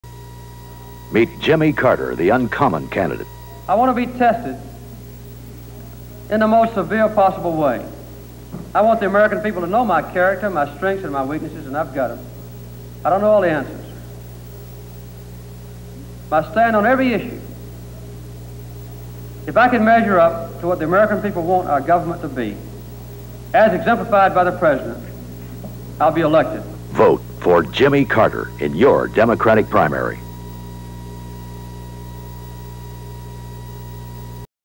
Jimmey Carter [Democratic] 1976 Campaign Ad "Tested"
Carter is shown giving a speech in which he says that he wants to be severely tested so that the American people can know his strength and weaknesses.        Duplicate--Original #51296

Creator: Magus Corporation

Location: United States of America

Format: 3/4” film; video/mp4

Extent: 34 seconds

Subjects: Positive Campaigning